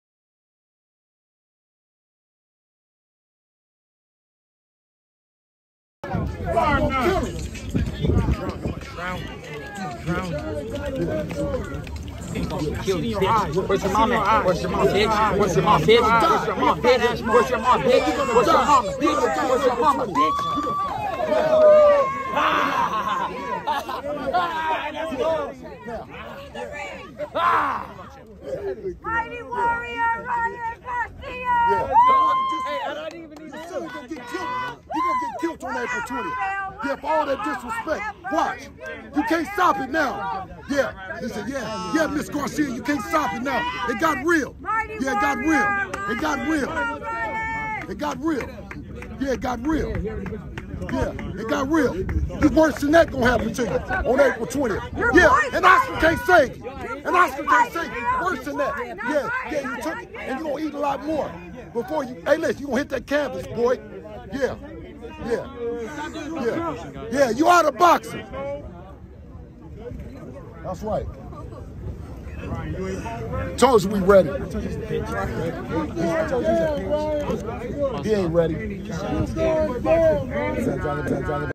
Devin Haney SMACKS & PUSHES Ryan Garcia in HEATED face off at Empire State as brawl almost happens!
Devin Haney smacks and pushes Ryan Garcia during their face off atop the empire state building as the two almost brawl! As the two fighters get in a heated situation ahead of their Haney vs Garcia fight

Devin Haney SHOVES Ryan Garcia! ALMOST BRAWL during face off at top of Empire State Building